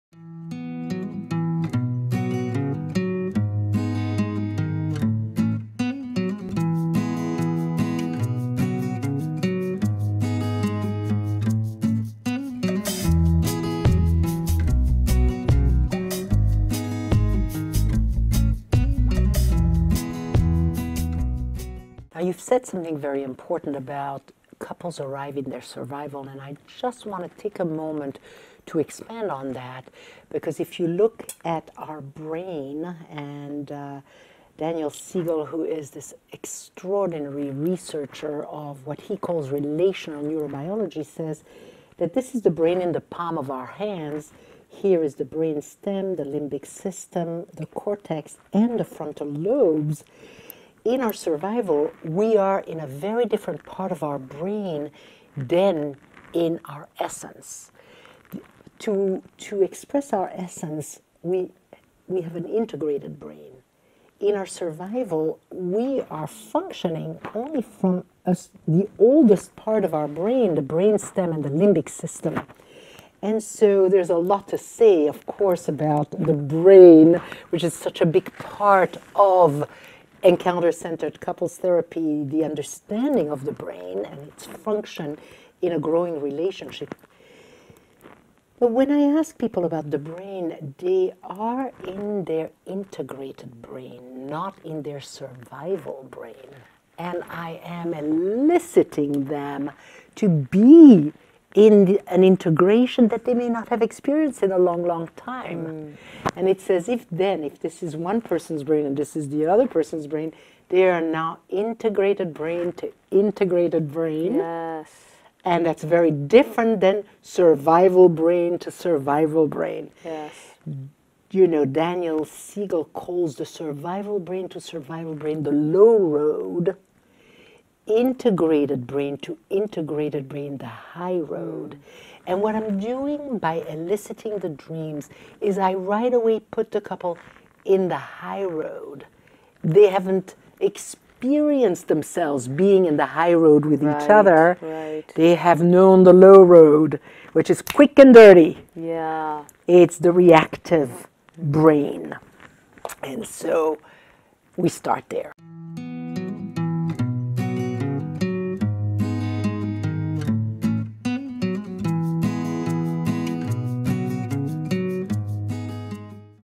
Hedy Schleifer: The neurobiology of "Taking the High Road"
In this clip, Hedy Schleifer uses her hands to demonstrate what parts of the human brain are used for survival thinking and what parts are used for expressing our essence. Our survival thinking, often reactionary and impulsive, uses the lower parts of the brain, as demonstrated by Hedy's wrist and thumb. The higher parts of our brain are used to express our true essence, represented by Hedy's fingers folding over her thumb. She explains that she teaches people in growing relationships to "take the high road" and engage each other with the higher parts of their brain, rather than a "take the low road" approach that leads to people engaging each other with the survival parts of their brain.